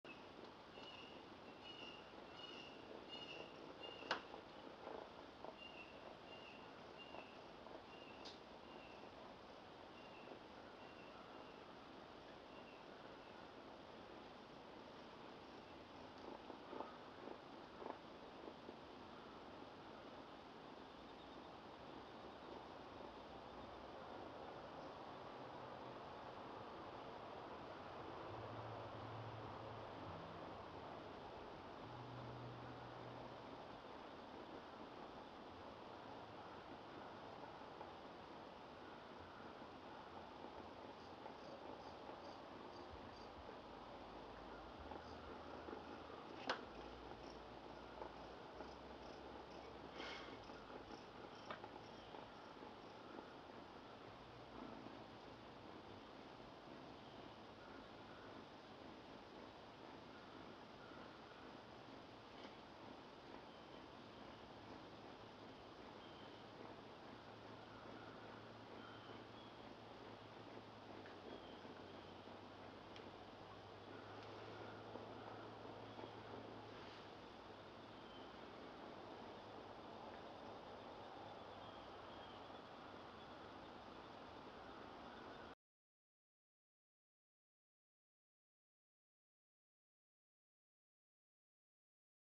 The Fox and The Dogfood
Nightly Visitor to my back yard.This female and a male.